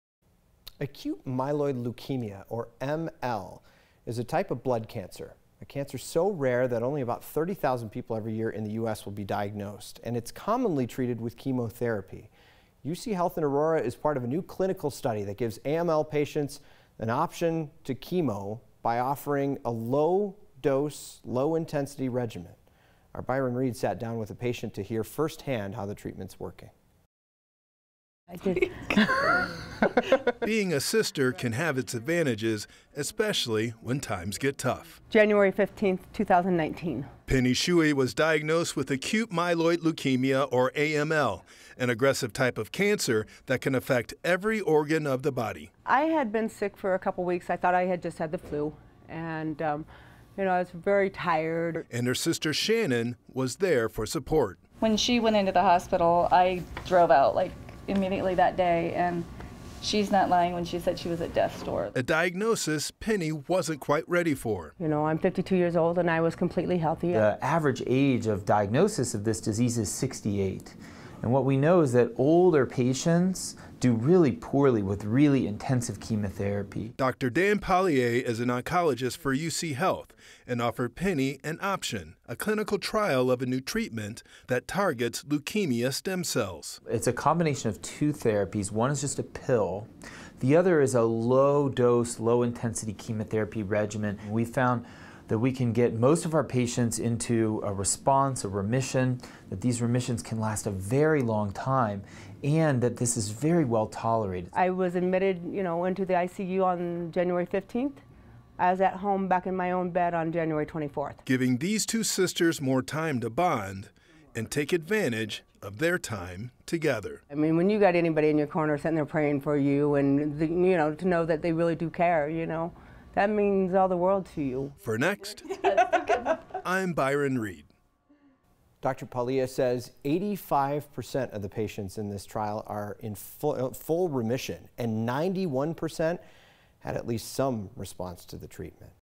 New drug may cure a strain of Leukemia
A new treatment appears to be more aggressive than the cancer it treats

9NEWS (KUSA) is located in Denver, Colorado.